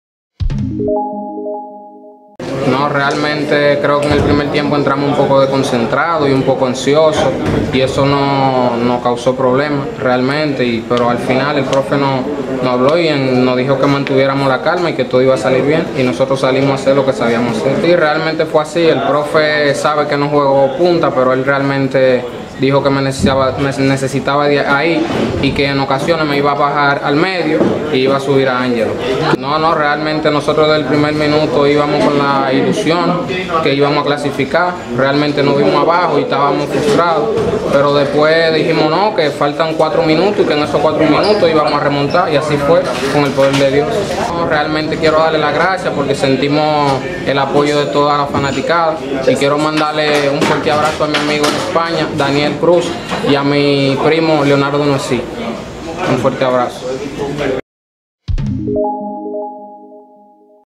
SEDOFÚTBOL SUB 20| ENTREVISTA JASON JOSEPH
Palabras del delantero Jason Joseph Yambatis luego de conseguir la clasificación a los octavos de final del Premundial Sub 20 Masculino de Concacaf 🎥⚽️🇩🇴🔥